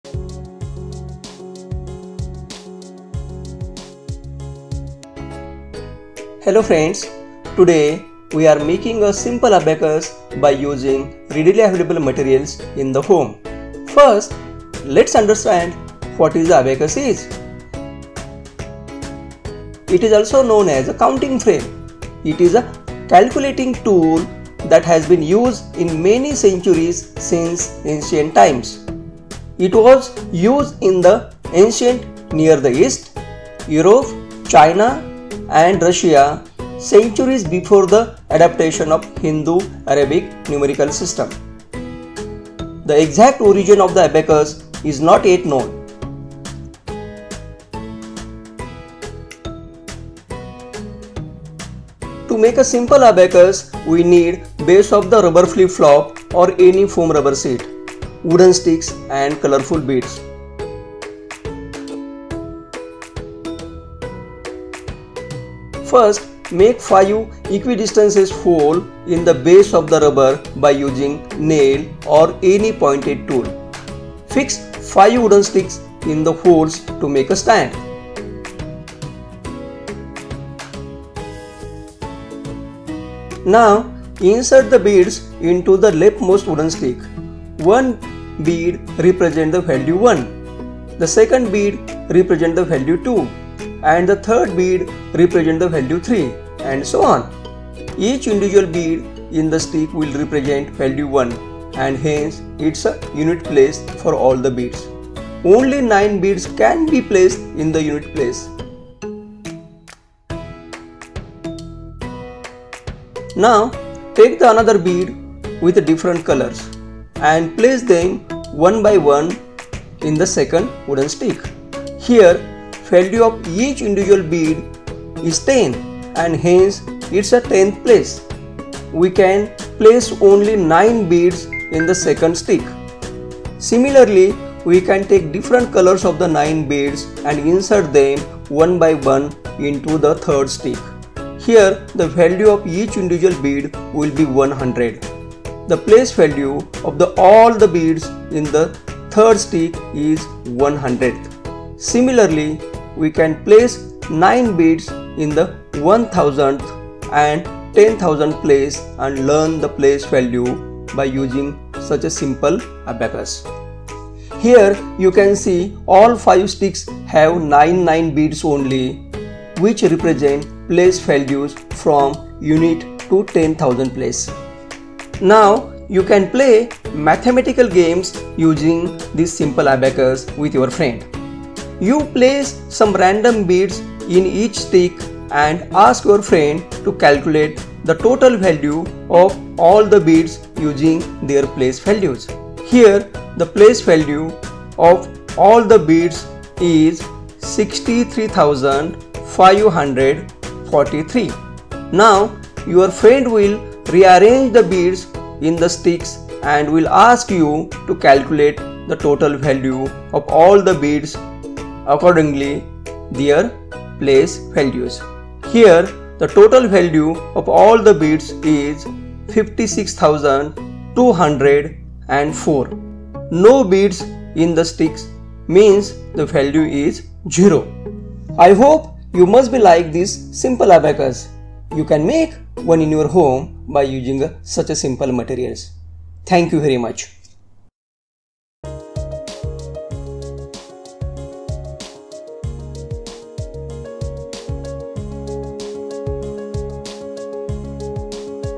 Simple Abacus | English
Here we are making a simple abacus by using readily available materials in the home and learning various activities of place values of the numbers.

Science Outreach Team: Prof. M. S. Santhanam, Dr Sourabh Dube, Dr Aparna Deshpande, Shubhangi Wankhede, Ashok Rupner, Dr Neeraja Dashaputre, Dr Chaitanya Mungi, Ankish Tirpude, Shraddha Bhurkunde, Dinesh Turkar, Pradnya Pujari Sakhee Bhure, Dr Apurva Barve, Dr Asim Auti, Shanti Pise, Shubham Patil and Rohit Piwal